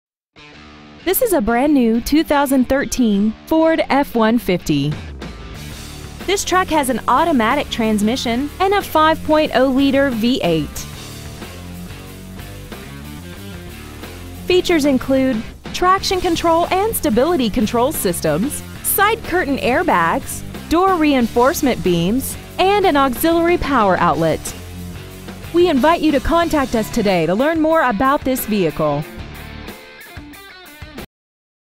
2013 FORD F-150 Houston TX
We have been honored to serve the Houston TX area , we promise that your experience at our dealership will exceed your expectations ! Year : 2013 Make : FORD Model : F-150 Engine : 5.0L V8 32V MPFI DOHC Trans . : 6-SPEED AUTOMATIC Exterior : BLUE Miles : 1 Interior : STEEL GRAY Stock : DFC34043 Champion Ford Gulf Freeway Gulf Freeway Houston , TX 77034